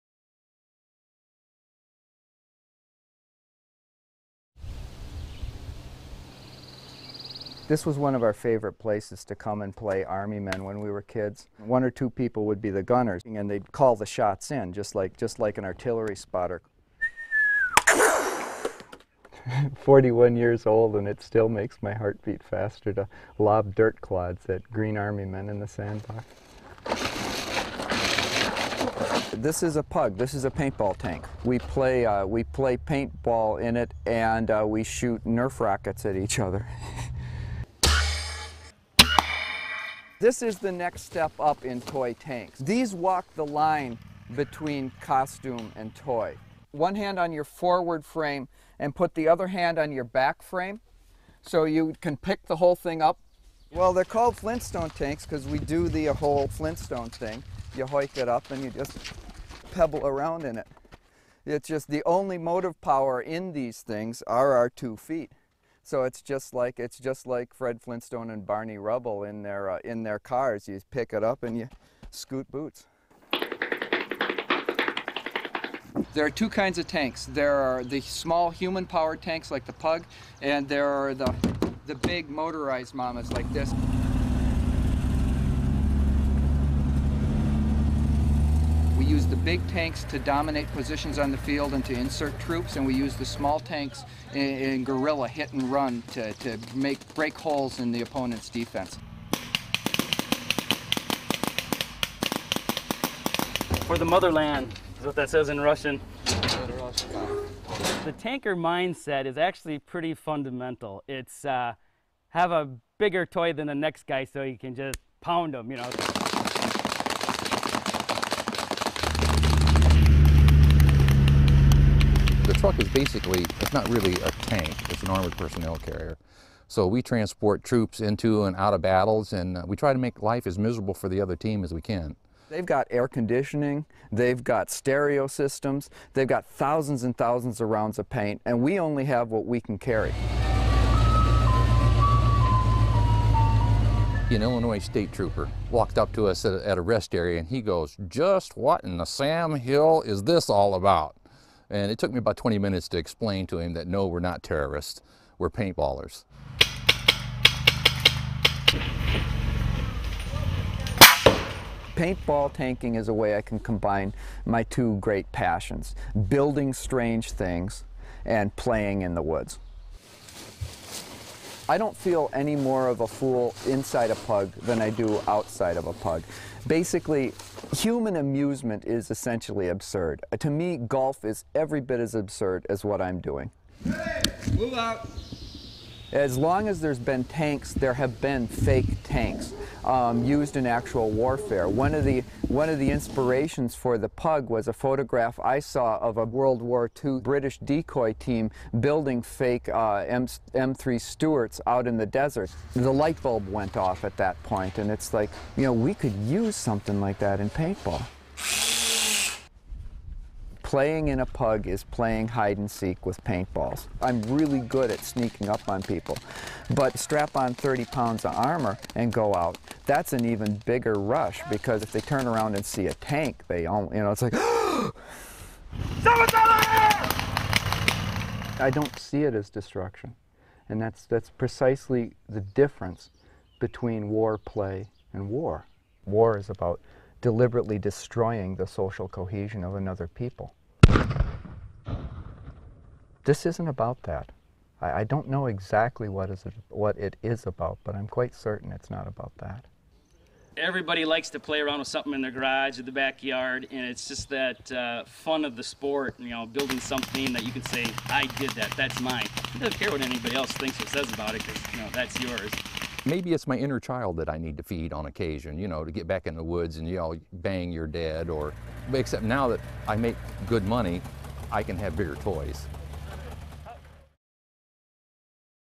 TOYING WITH WAR: Tank Commander
-- What if the Flintstones had armored tanks?  The people of Bedrock are a peaceful lot, but these "PUG" walking tanks would be ideal for Fred and Barney to let out some pent-up aggression.

Wanna learn more about the PUG tank inventor and how you can make it part of your paintball wars?